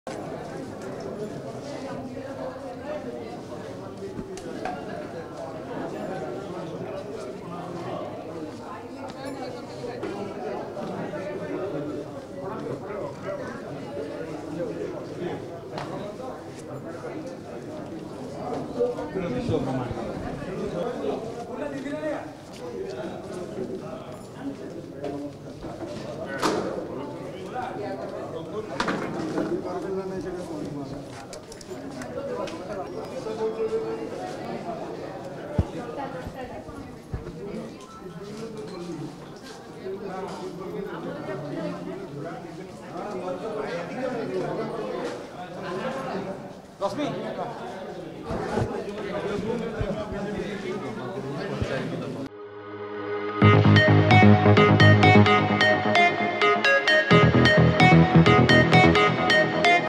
ସାର୍.. ଟିକେ ପଣା ପିଅନ୍ତୁ | Odisha Reporter
ମହା ବିଷୁଭ ସଂକ୍ରାନ୍ତିରେ ପଣା ପିଇଲେ ରାଜ୍ୟ ବିଜେପି ସଭାପତି ମନମୋହନ ସାମଲ । ଦେଖନ୍ତୁ ସେହି ସମୟର ଚିତ୍ର  ।